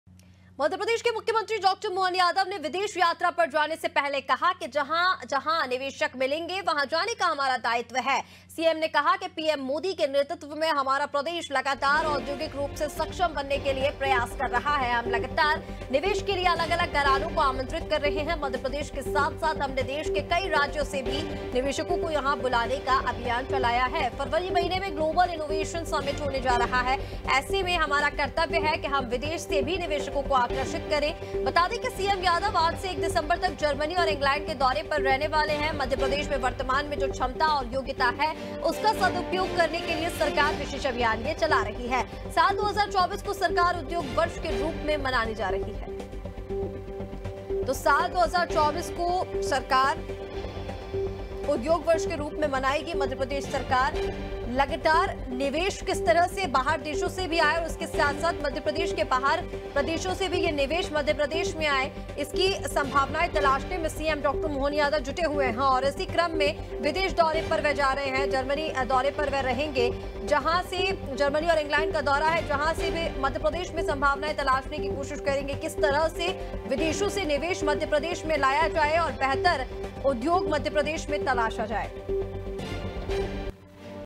Madhya Pradesh : MP में विदेश से आएगा निवेश | MP Hindi News || Anaadi Tv
मध्य प्रदेश के मुख्यमंत्री मोहन यादव ने विदेश यात्रा पर जाने से पहले कहा कि, जहां जहां निवेशक मिलेंगे वहां जाने का हमारा दायित्व है...सीएम ने कहा कि, पीएम मोदी के नेतृत्व में हमारा प्रदेश लगातार औद्योगिक रूप से सक्षम बनने के लिए प्रयास कर रहा है... हम लगातार निवेश के लिए अलग-अलग घरानों को आमंत्रित कर रहे हैं...मध्य प्रदेश के साथ-साथ हमने देश के कई राज्यों से भी निवेशकों को यहां बुलाने का अभियान चलाया है...फरवरी महीने में ग्लोबल इनोवेशन समिट होने जा रहा है...ऐसे में हमारा कर्तव्य है कि हम विदेश से भी निवेशकों को आकर्षित करें...बता दें, सीएम यादव आज से 1 दिसंबर तक जर्मनी और इंग्लैंड के दौरे पर रहेंगे... मध्य प्रदेश में वर्तमान में जो क्षमता और योग्यता है उसका सदुपयोग करने के लिए सरकार विशेष अभियान चला रही है...साल 2024 को सरकार उद्योग वर्ष के रूप में मनाने जा रही है...

HATHWAY- 231
SITI- 356
SKY- 441
ACN- 231
AIRTEL - 379
JIO TV, MX PLAYER, VI